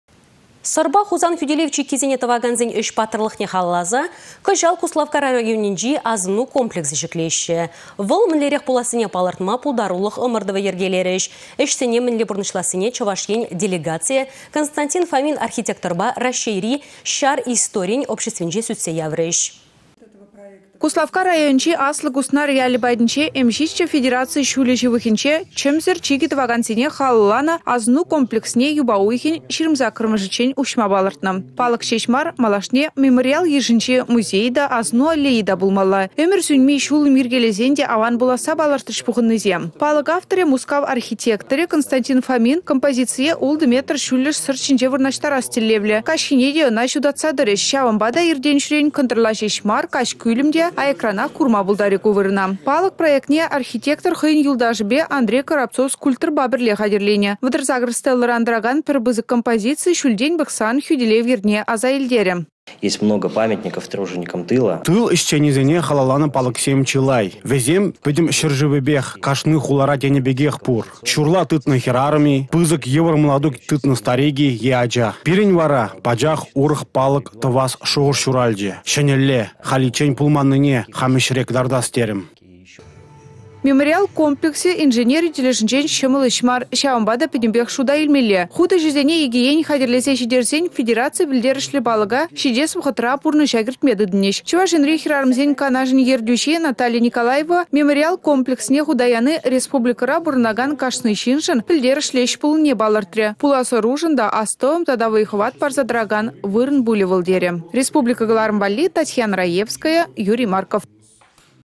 Федераци пĕлтерĕшлĕ палăк
Сăрпа Хусан хӳтĕлев чикккисене тăвакансен ĕç паттăрлăхне халалласа кăçал Куславкка районĕнче асăну комплексĕ çĕклеççĕ. Вăл мĕнлерех пулассине палăртма пултарулăх ăмăртăвĕ йĕркелерĕç. Ĕçене мĕнле пурнăçлассине Чăваш Ен делегацийĕ Константин Фомин архитекторпа Раççейри çар историйĕн обществинче сӳтсе яврĕ.